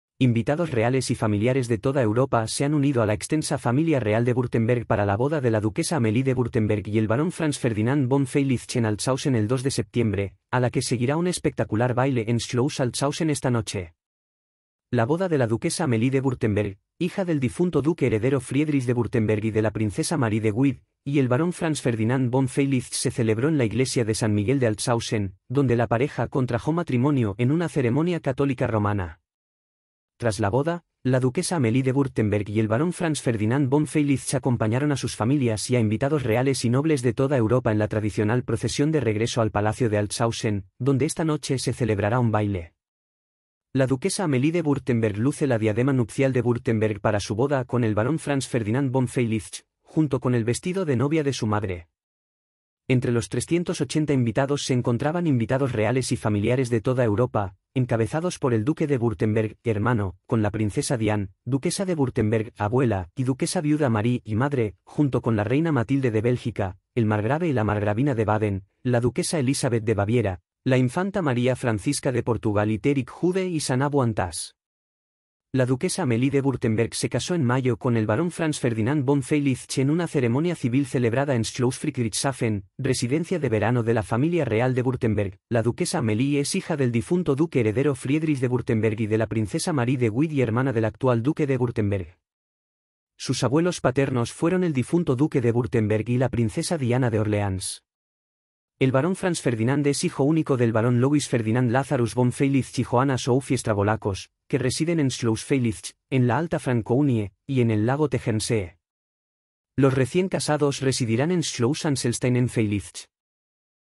Boda de la duquesa Amelia de Wurtemberg y el barón Franz-Ferdinand von Feilitzsch
Invitados reales y familiares de toda Europa se han unido a la extensa Familia Real de Wurtemberg para la boda de la duquesa Amelie de Wurtemberg y el barón Franz-Ferdinand von Feilitzsch en Altshausen el 2 de septiembre, a la que seguirá un espectacular baile en Schloss Altshausen esta noche.